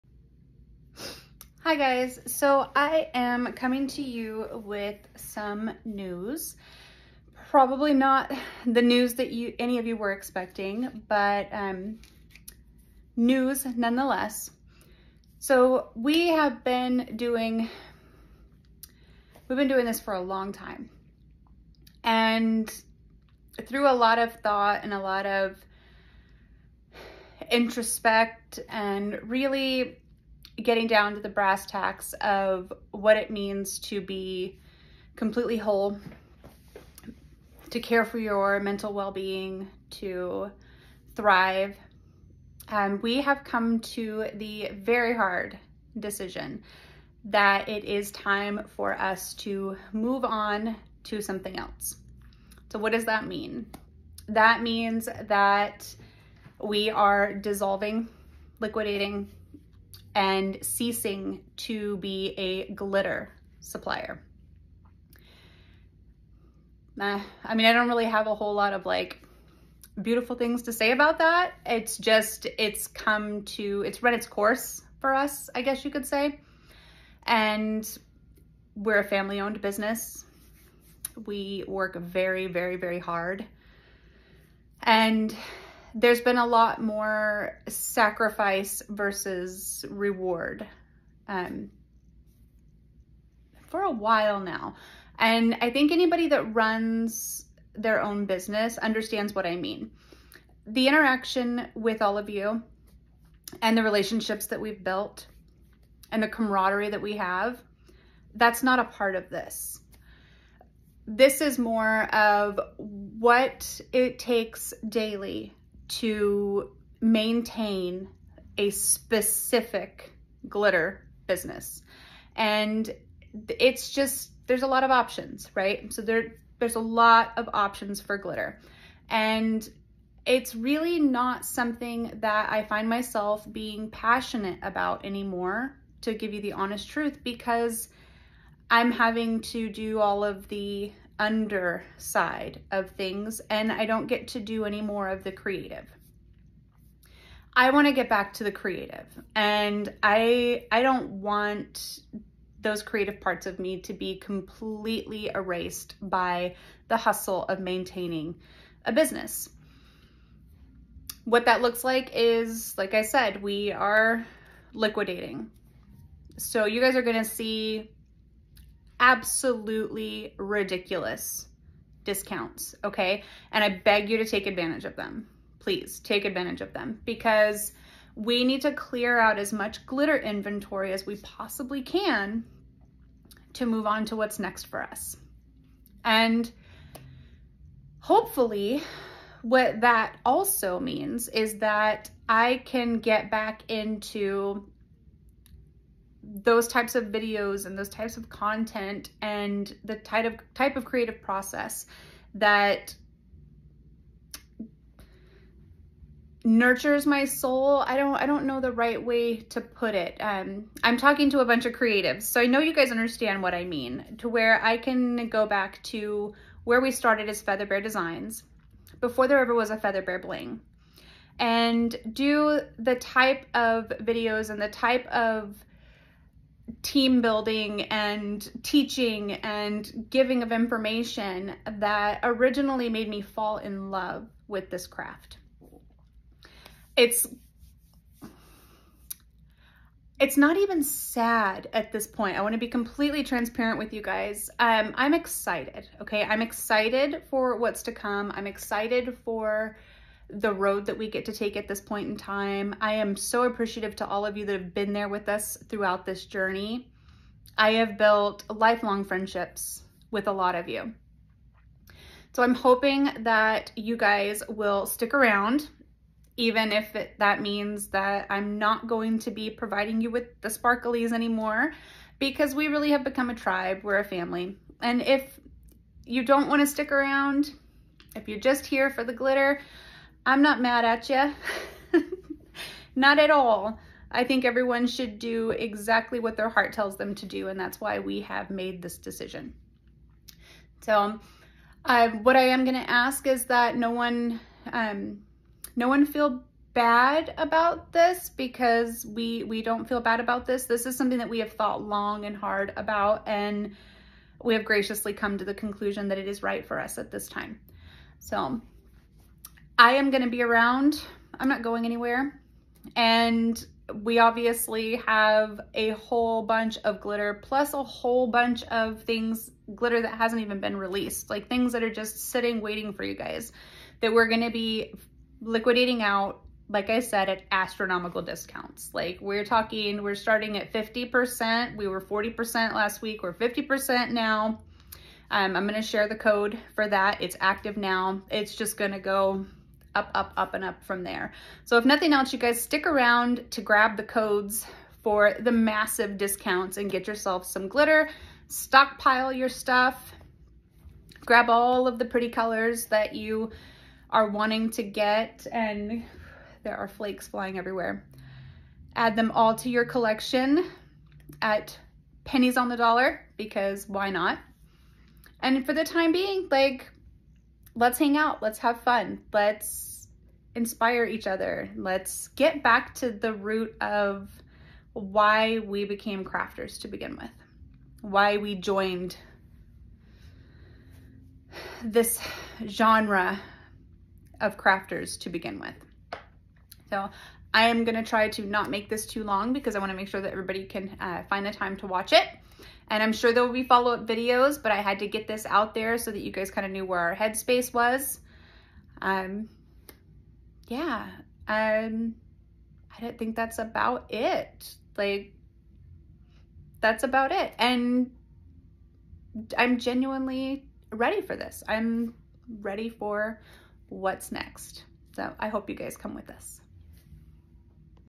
We're closing a chapter, to start a new book.
We have had the absolute best experience with all of you over all these years, Thank you so much for your unwavering support. FeatherBear Bling has made the decision to stop selling glitter, We are liquidating our inventory at insane discounts.  Use code LIQUID on all glitter purchases made starting from when you see this video. We cant thank you all enough for riding the wave with us and hope you know we consider you part of our family!